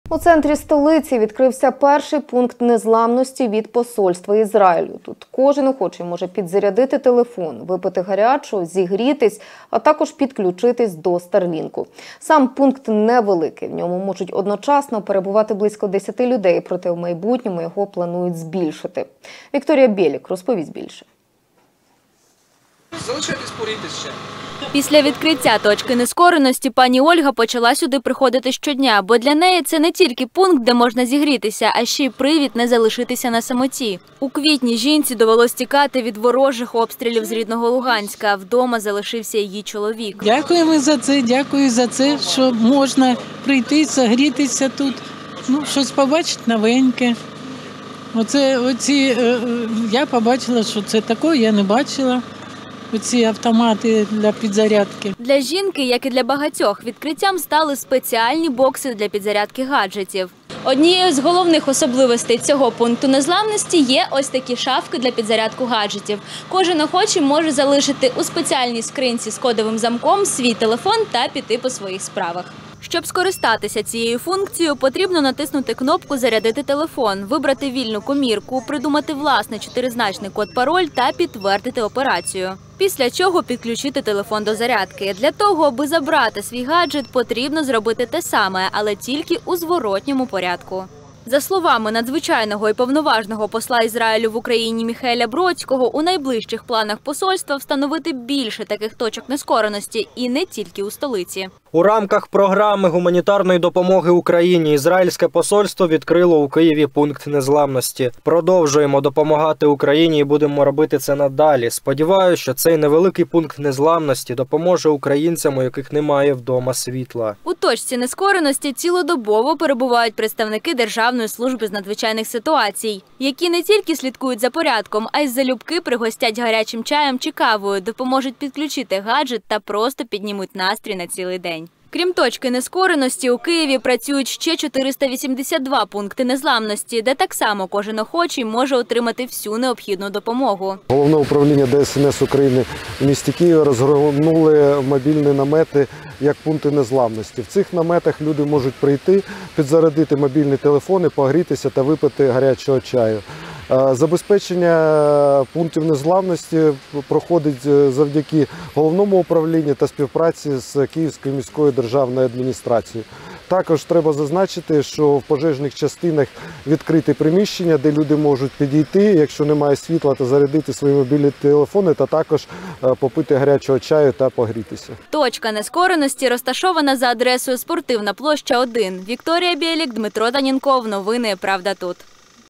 У столиці відкрили перший «пункт незламності» від Ізраїлю
У центрі Києва відкрився перший пункт незламності від посольства Ізраїлю. Тут кожен охочий може підзарядити телефон, випити гарячого, зігрітися, а також підключитись до Starlink. Сам пункт невеликий: у ньому можуть одночасно перебувати близько 10 людей, проте у майбутньому його планують збільшити. За словами надзвичайного і повноважного посла Ізраїлю в Україні Міхаеля Бродського,  у найближчих планах посольства, встановити більше таких точок нескореності, і не тільки у столиці.  Крім точки нескореності - у Києві  працюють ще 482 пункти незламності, де так само кожен охочий може отримати всю необхідну допомогу.